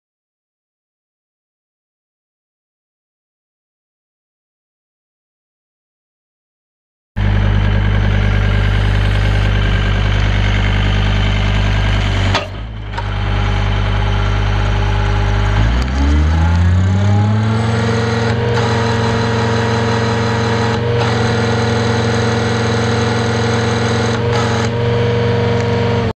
Danuser EP15 hydraulic post auger for sale | no-reserve Internet auction November 15, 2017
View a detailed listing of this item and place bids

Danuser EP15 hydraulic post auger
2" hex head
Forward and reverse
Operators manual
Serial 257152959
It's located in Morrill, KS, and we're selling it to the highest bidder regardless of price.